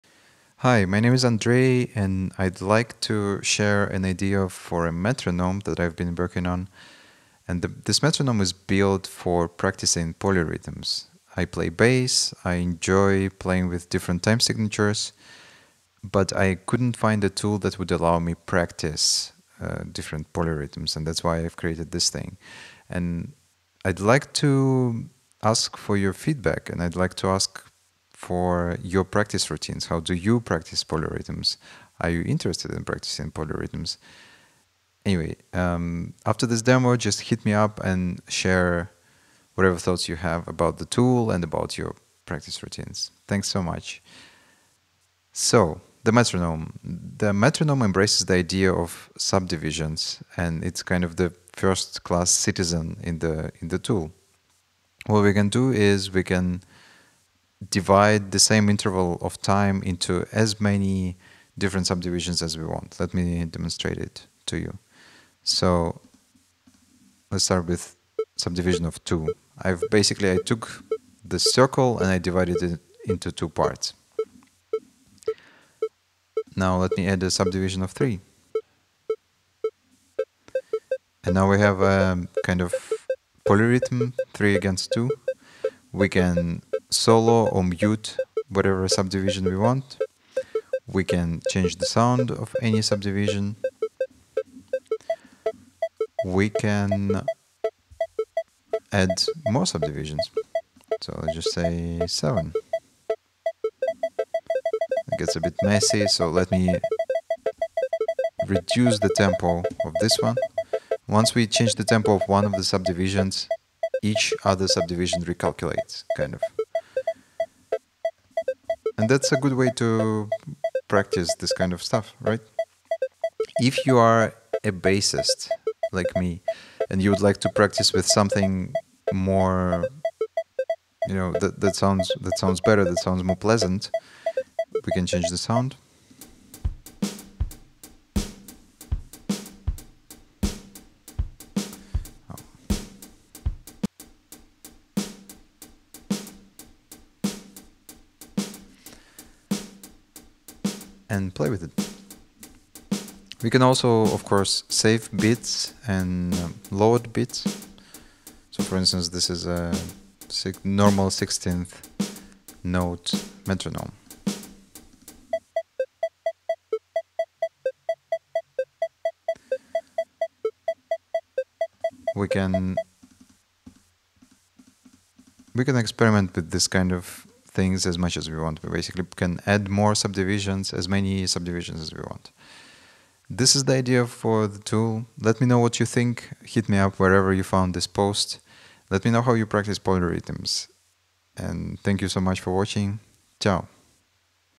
Polyrhythm metronome demo
This is a prototype for a metronome that's built specifically to practice polyrhythms. Please let me know what's your practice routine and what you think of the tool in the comments.

The project is now live and accessible for free on polychops.com